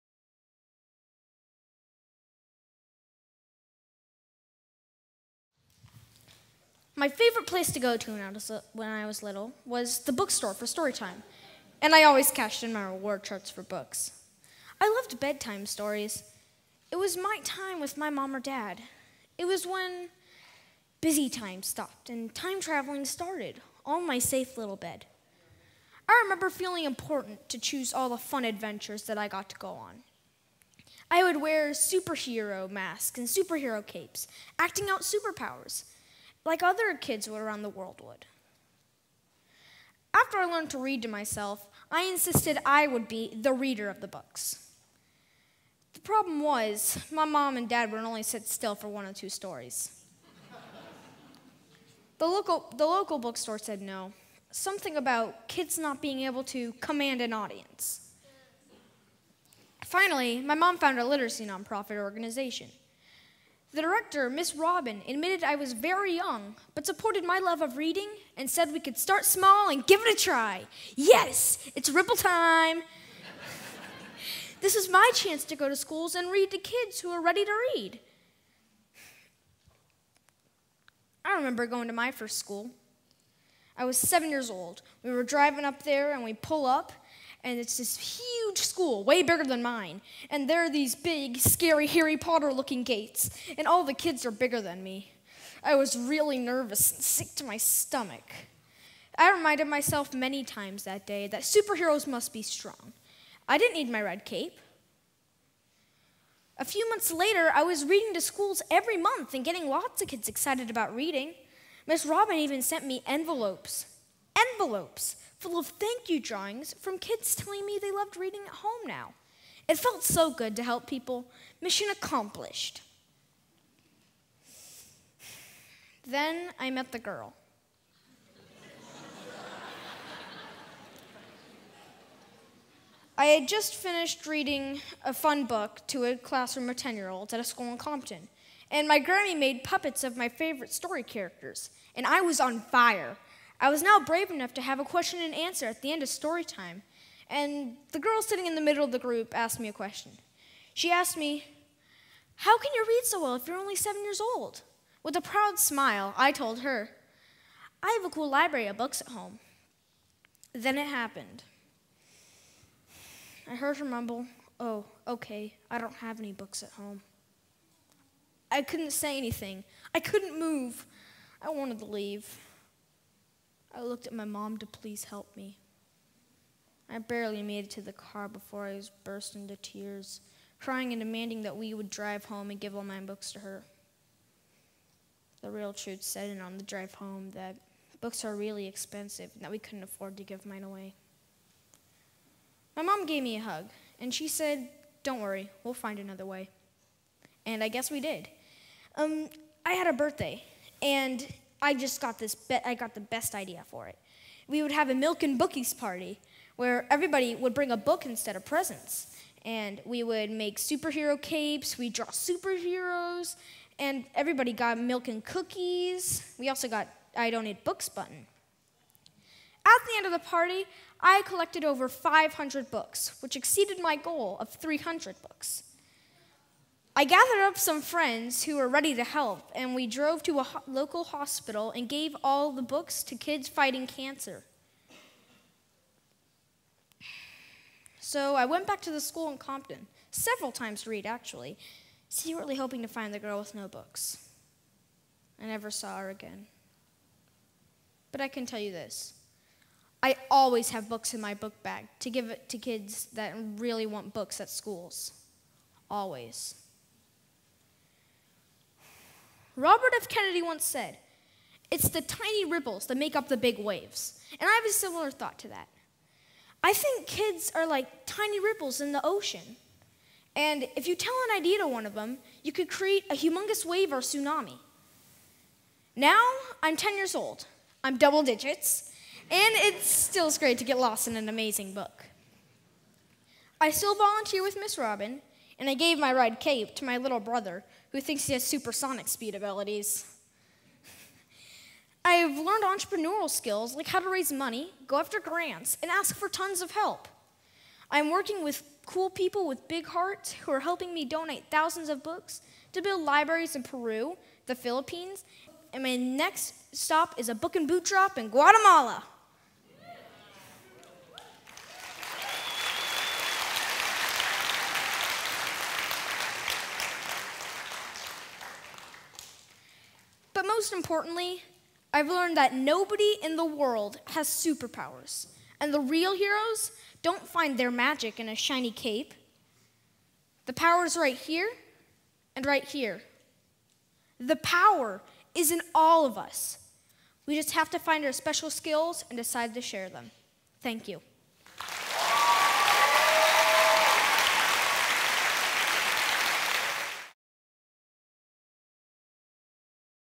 Kid Philanthropism 101: Gabe Eggerling at TEDxYouth@BommerCanyon
What do you do when you discover the rest of the world doesn't have access the same resources you do? Well, duh. You go change the world. It's sort of just that simple to junior philanthropist, Gabe Eggerling. His passion is reading, but his world was rocked when he learned that most kids in the world can't read simply because they have no books. This inspiring tale of action will inspire and empower anyone who's ever considered giving back, and after watching Gabe speak, you will never look at a book, library (or philanthropy for that matter) the same way again.